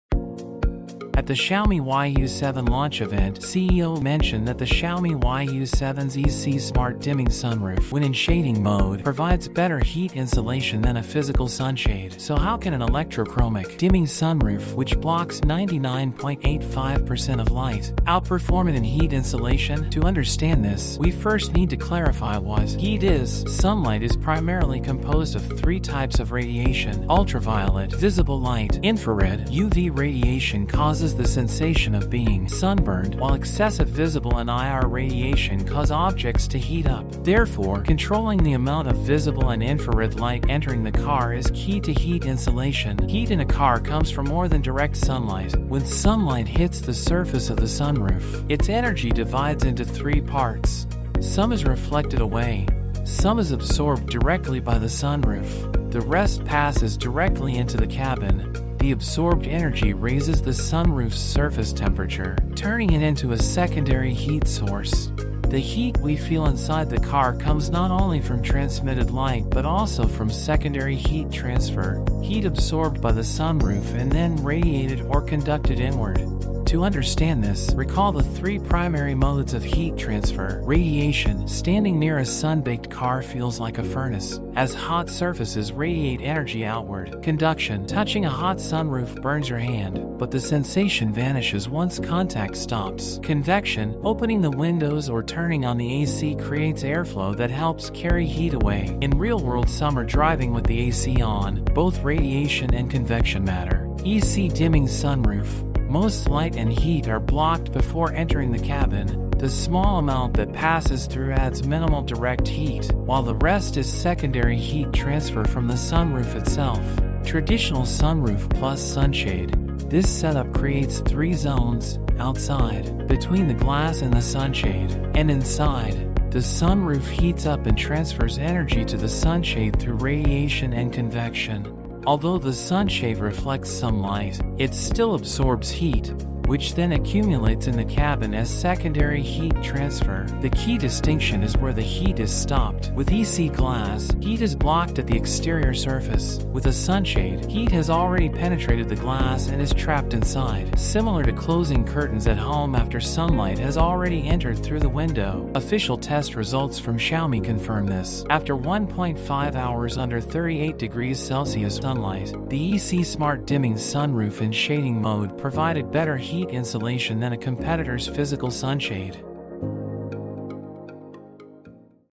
Smarter Heat Blocking: EC Sunroof vs. Traditional Shade
How can a see-through sunroof block heat better than a physical sunshade?
In this video, we break down the science of heat insulation and explore why EC smart dimming technology represents a major advancement for in-car comfort.
Watch to learn the science of thermal insulation and why EC technology is the next step in sunroof design.